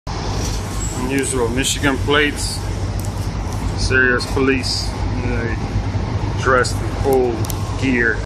WTF?!?! What's with the presence?
How, why or what reason could explain a LawEnforcement vehicle from Michigan with giant LapTop between driver/passenger, cage separates rear seats, passenger in full out SWAT/SEAL gear in Los Angeles?